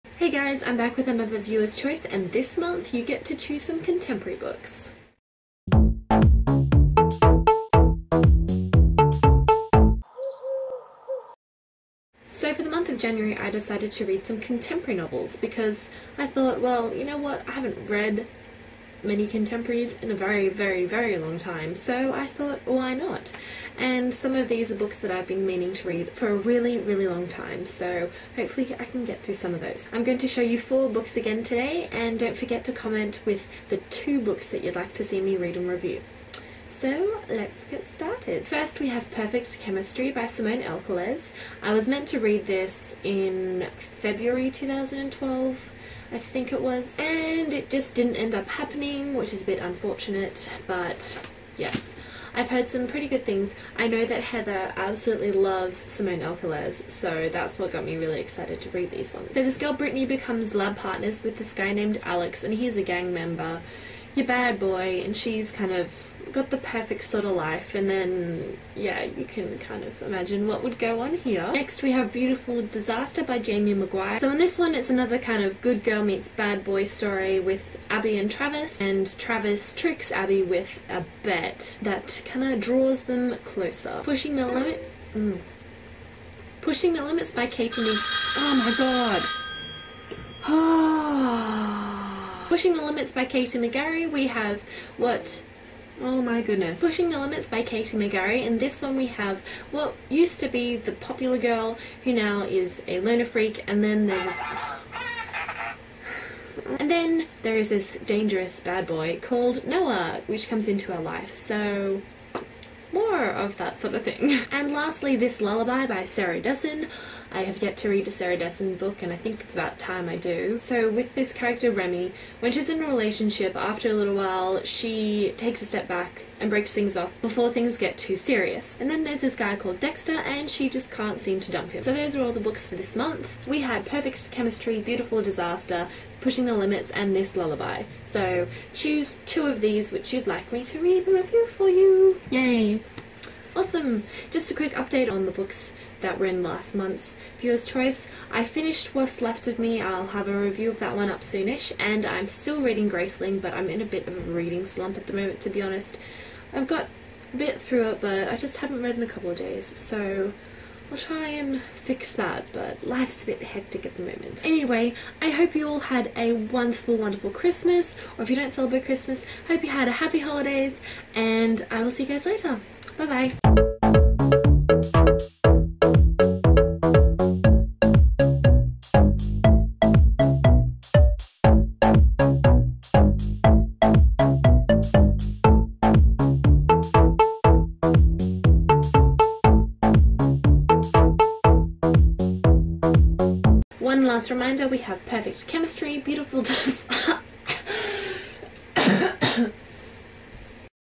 January Viewers Choice [VOTING CLOSED]
Comment with the 2 of these you would like to see me read and review :)

1. Perfect Chemistry by Simone Elkeles
2. Beautiful Disaster by Jamie McGuire
3. Pushing the Limits by Katie McGarry
4. This Lullaby by Sarah Dessen